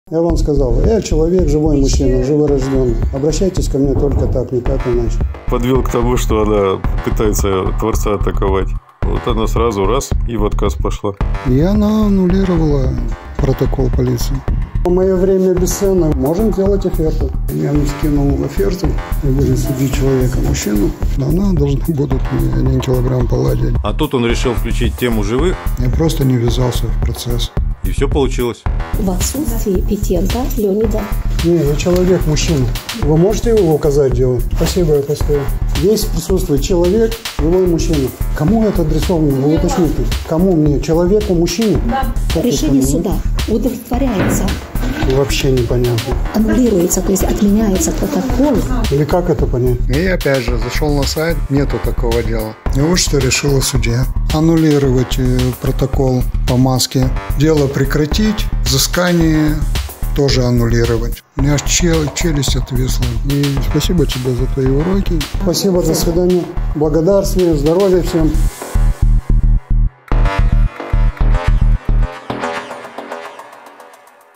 Трейлер ЖИВОЙ МУЖЧИНА ИЗ МОЛДАВИИ или ОТВОД УДОВЛЕТВОРЁН Часть 2 2022.12.05 Молдова судья полиция
на ты или на вы?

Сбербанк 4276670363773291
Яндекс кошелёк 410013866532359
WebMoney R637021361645